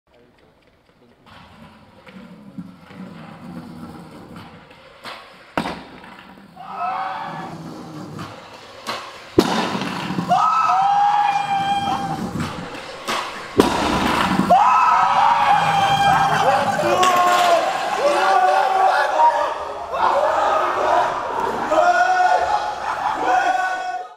KRISTIAN ALBRECHT - WELCOME PRIME SCOOTERING
The CZ beast Kristian Albrecht just made history by being the first scooter rider to land the Lyon 25. Welcome to the team bro !

Filming: Antoine Baldisserri / Maxime Bouzid
Editing: Antoine Baldisserri